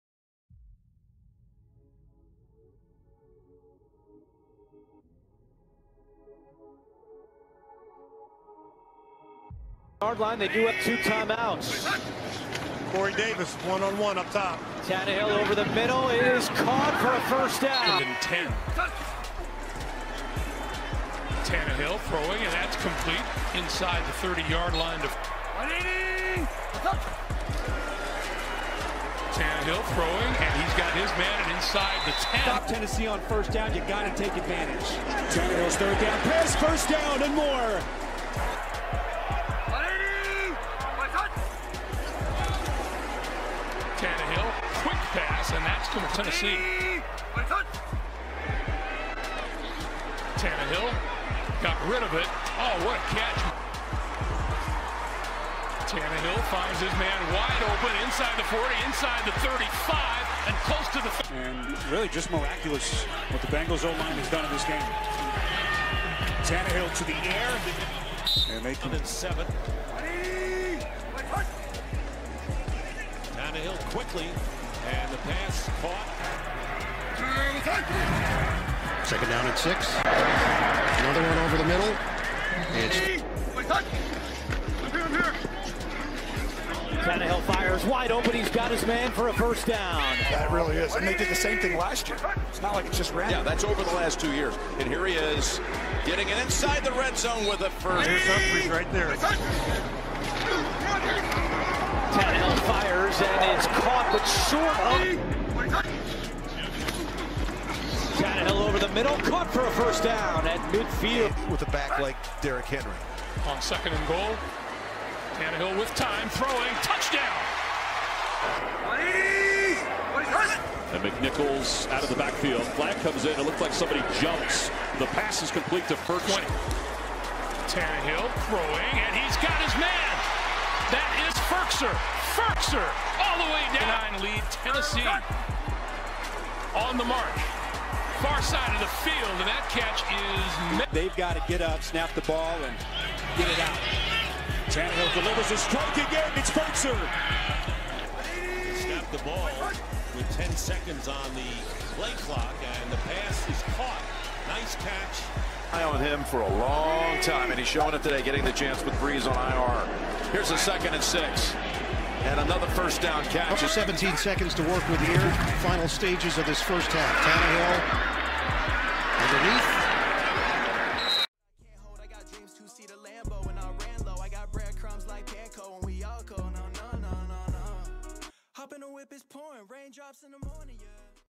ATLANTA FALCONS: Anthony Firkser ᴴᴰ
BREAKING NEWS || ATLANTA FALCONS || ANTHONY FIRKSER HIGHLIGHTS
58 CAREER GAMES
106 RECEPTIONS
1,107 YARDS
5 TOUCHDOWNS